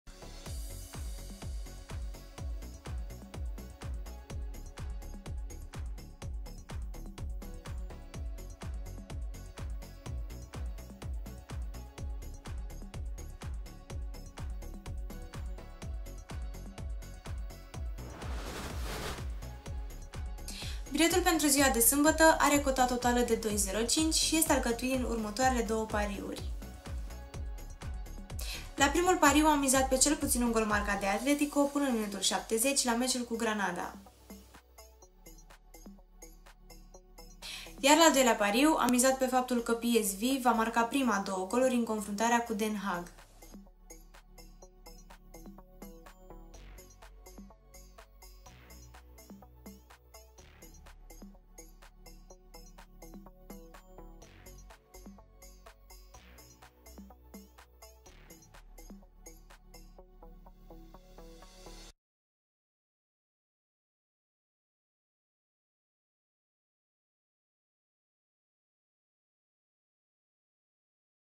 Biletul Zilei COTA 2 - 13.02.2021
Biletul zilei COTA 2 la pariuri pentru sambata, 13 februarie, propus de Cristina este este reprezentat de un singur pariu cu cota de 2.05.